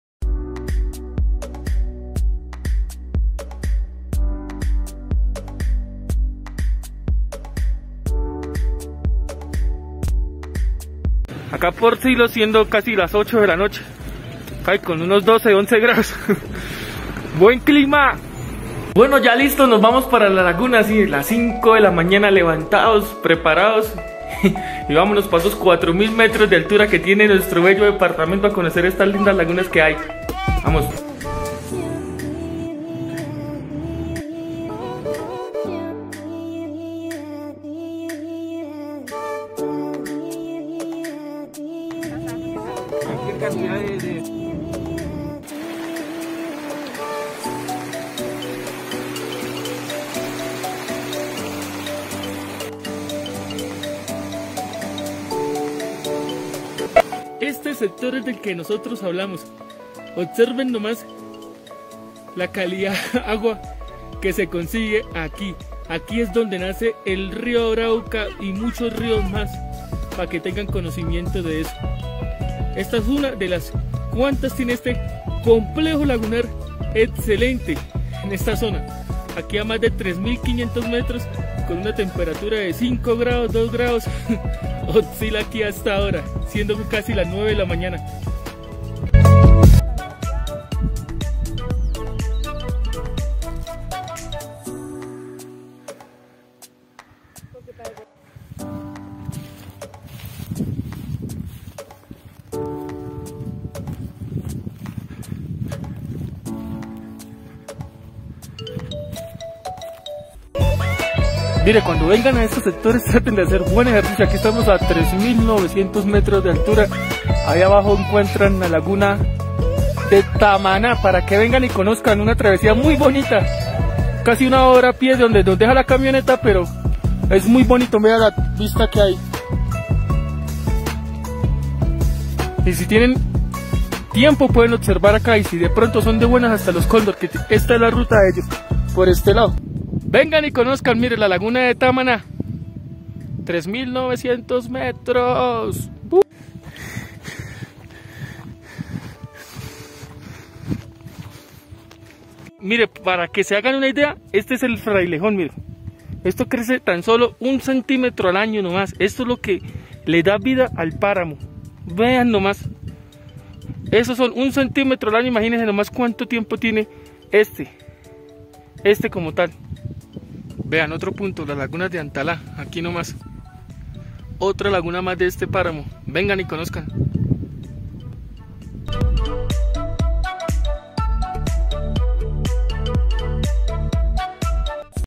les gustaría conocer este lugar - Lagunas del Municipio de Silos - Paramo de Santurbán
les gustaría conocer este lugar - Lagunas del Municipio de Silos - Paramo de Santurbán (hay que volver a respirar aire puro)

este recorrido lo realice en el año 2021, fue una experiencia unica la cual debo de volver hacer porque nuestro departamento es muy rico por vamos a retomar estas salidas en el año 2024 a lugares que deben de conocer por lo menos una vez, a compartir amigos y reacciones y que opinan ustedes les gustaria conocer este lugar.